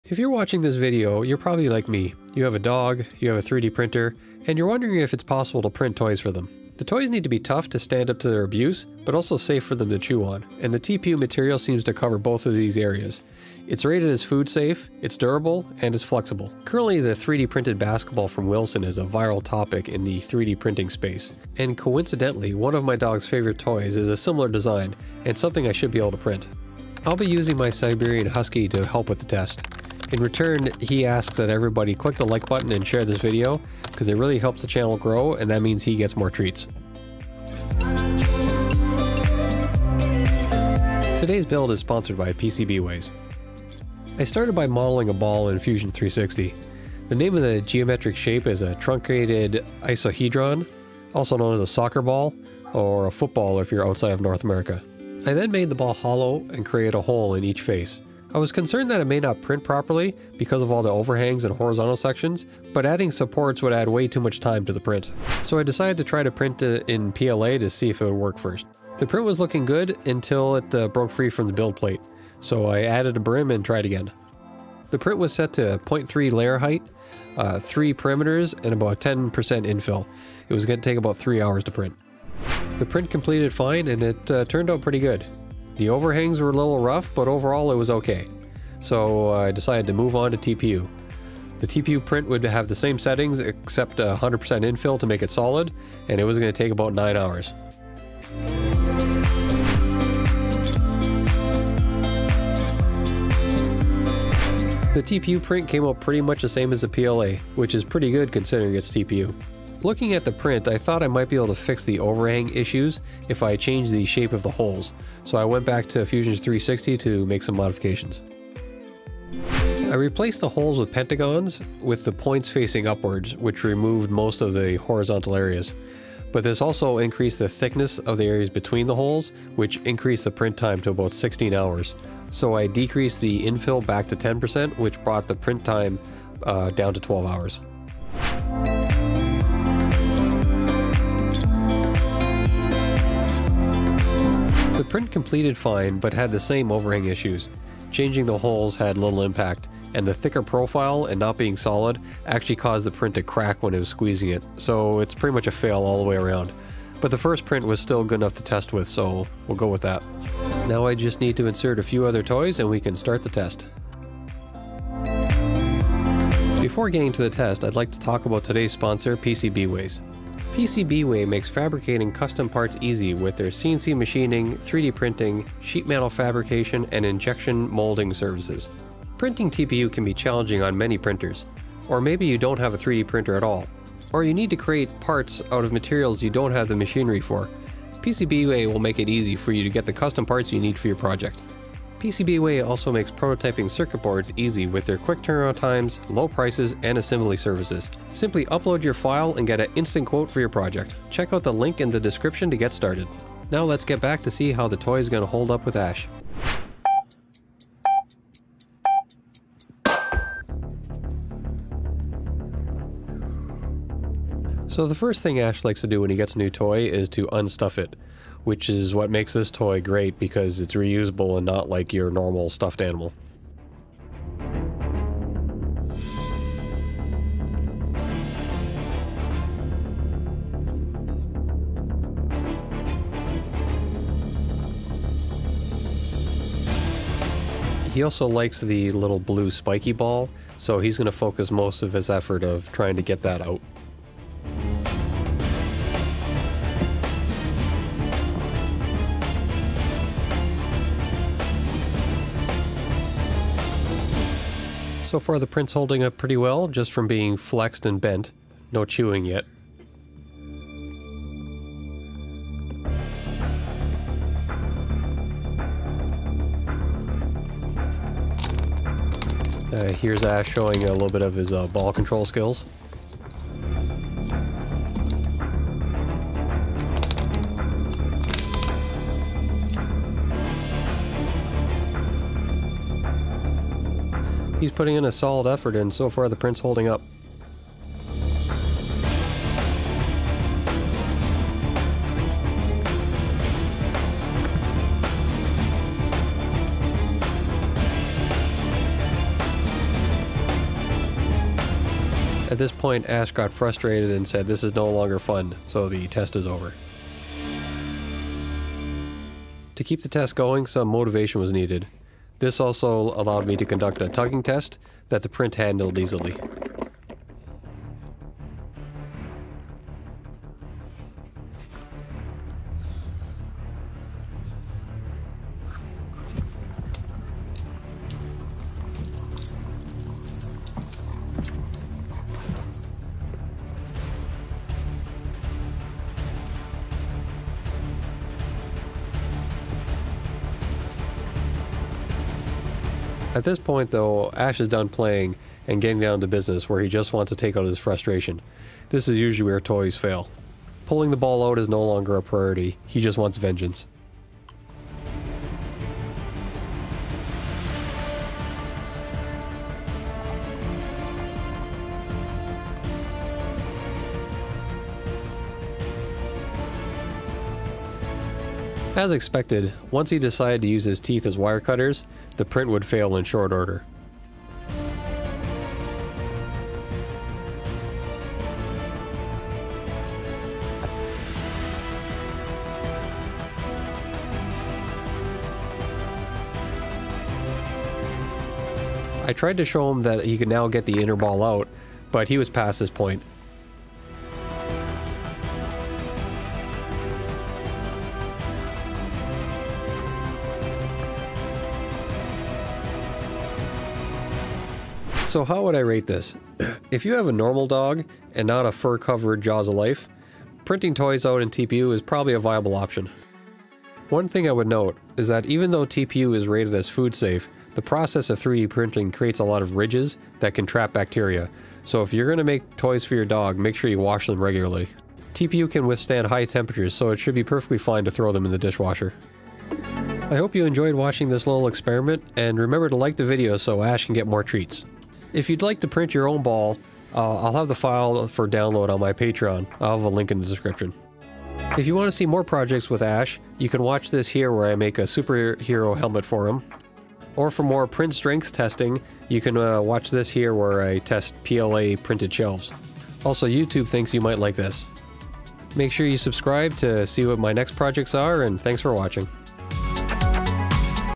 Creating a 3d Printed Dog Toy in TPU and testing its strength against a Husky
Watch as I try 3D printing a dog toy using flexible materials! I'll go through the steps designing the toy and then printing it with TUP filament. Then we'll give the toy to my husky to play with and see how it hold up.

Chapters:
Intro: 0:00
Modeling and Printing: 0:44
Testing: 3:26
Results: 6:45

⚡ items specific to this build:
Some TPU options.

▶️ iPhone Pro Max 15

▶️ Phone to SSD USB-C cable